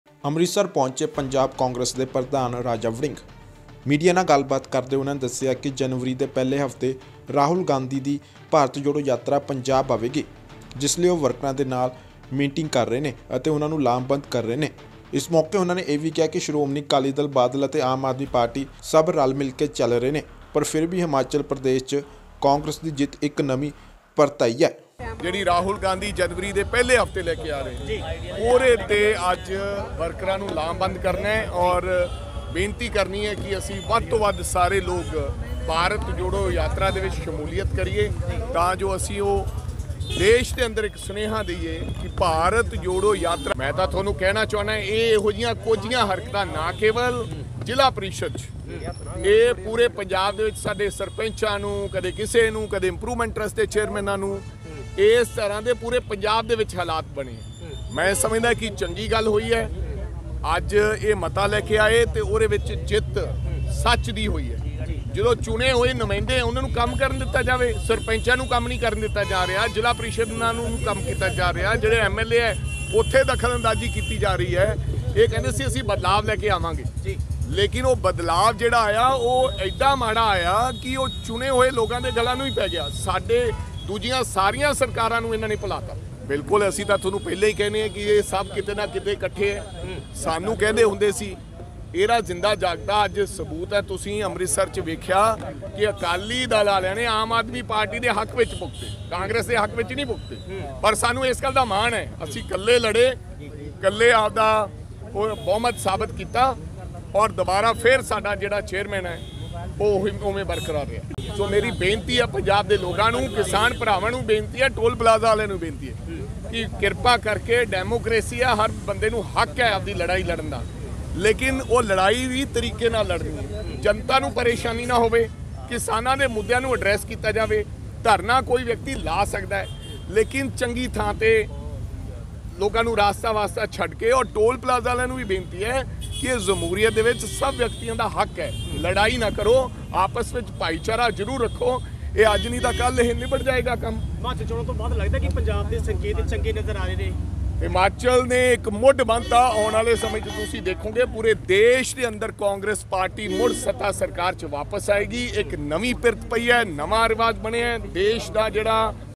ਅੰਮ੍ਰਿਤਸਰ ਪਹੁੰਚੇ ਪ੍ਰਧਾਨ ਰਾਜਾ ਵੜਿੰਗ, ਅਕਾਲੀ ਦਲ ਤੇ ਆਪ ਪਾਰਟੀ ਤੇ' ਸਾਧੇ ਨਿਸ਼ਾਨੇ | Bharat Jodo Yatra Punjab
ਅੰਮ੍ਰਿਤਸਰ ਪਹੁੰਚੇ ਪ੍ਰਧਾਨ ਰਾਜਾ ਵੜਿੰਗ, ਅਕਾਲੀ ਦਲ ਤੇ ਆਪ ਪਾਰਟੀ ਤੇ' ਸਾਧੇ ਨਿਸ਼ਾਨੇ 👇